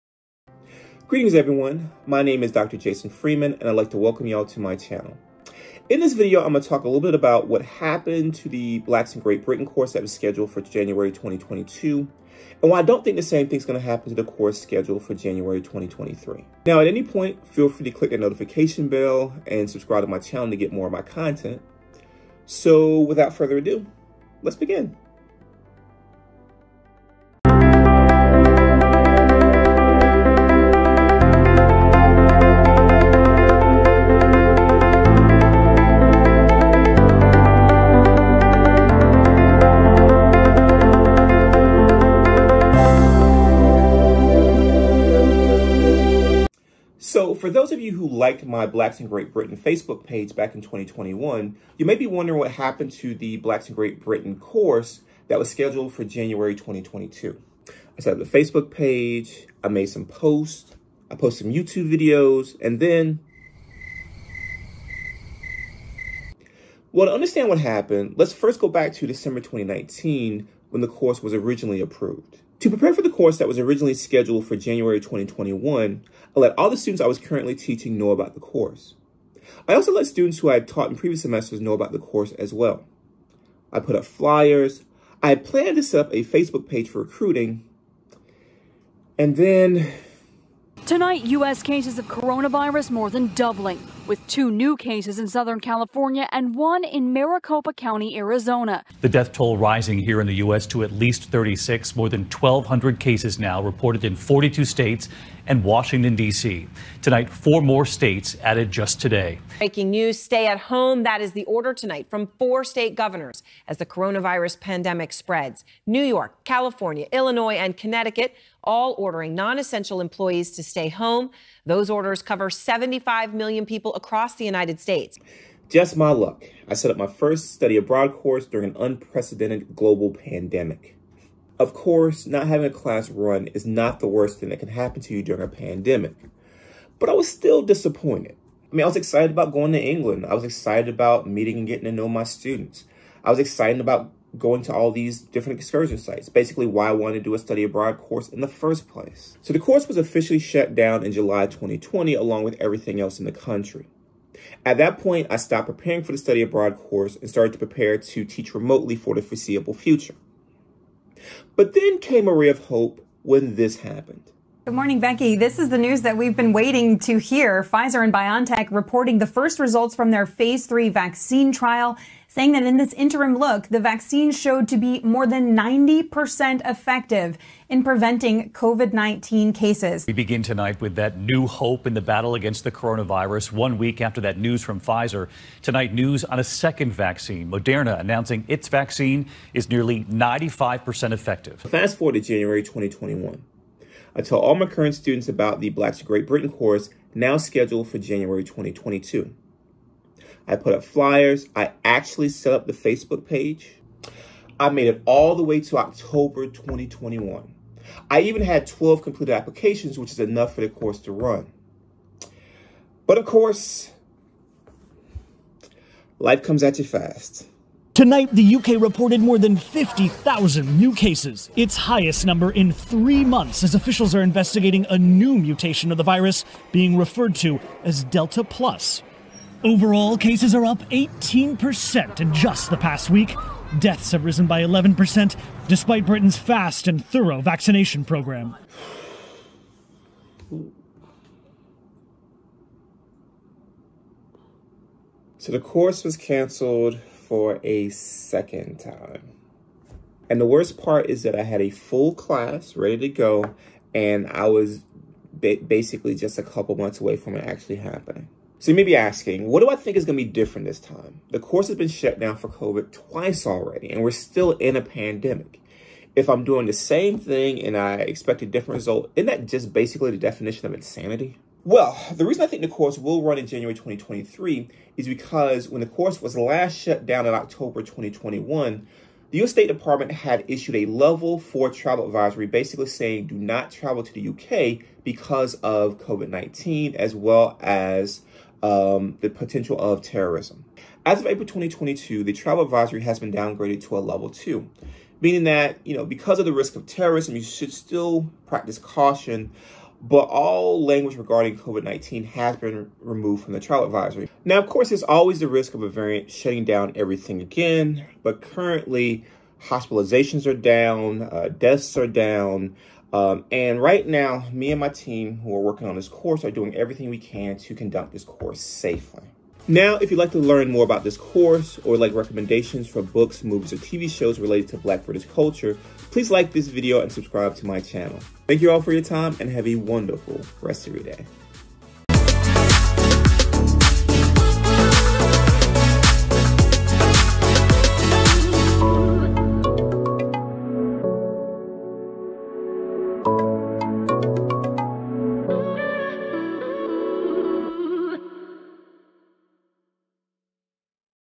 So, What Happened with the January 2022 Course?
In this video I reveal what happened with my Blacks in Britain Study Abroad Course that was scheduled for January 2022.

Towson University